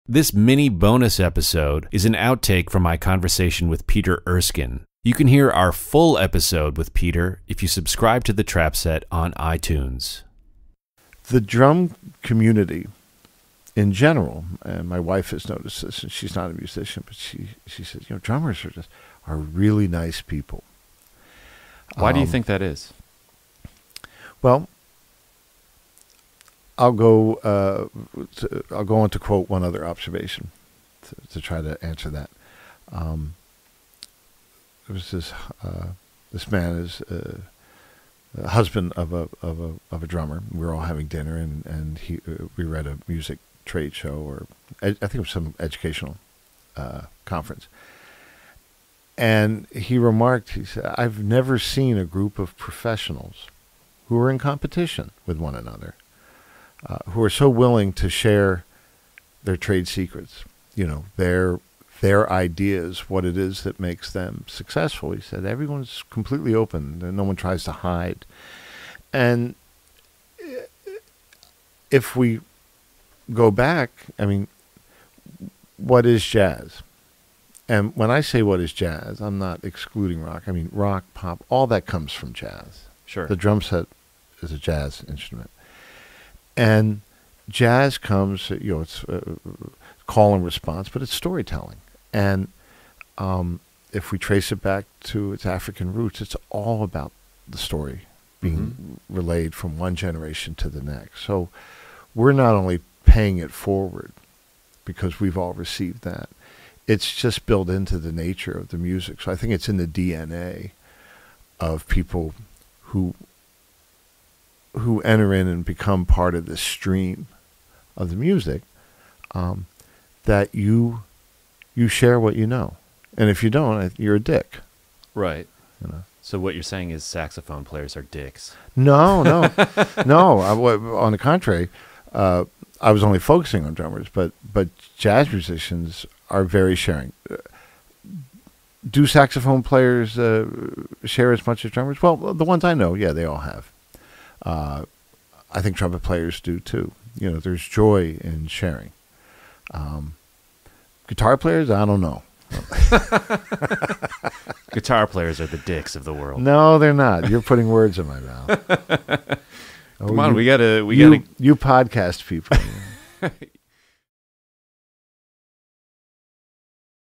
BNS: Peter Erskine on Generosity
Peter Erskine talks about generosity among drummers and jazz musicians in general. Don't be a dick, share your knowledge.